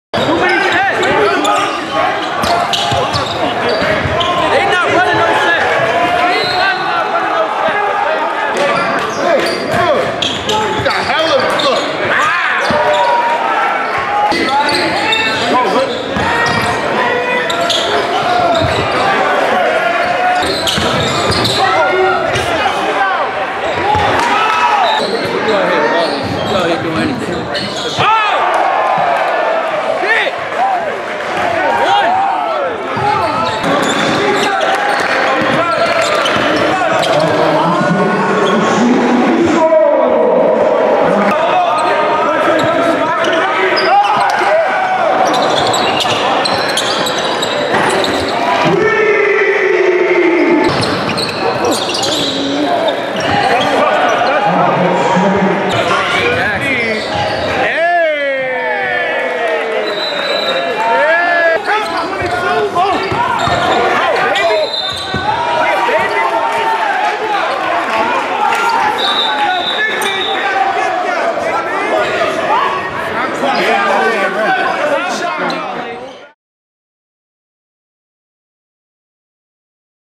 New Recruit Media LLC presents: 6’4 G OTEGA OWEH 2022
6’4 G OTEGA OWEH 2022

@_otegaoweh has the heart of a warrior, OTEGA leads with his actions. The kid doesn’t take plays off and he is relentless around the basket. OWEH has rare athleticism, he can turn it on at any moment but the kid is also a SNIPER from deep.

Check out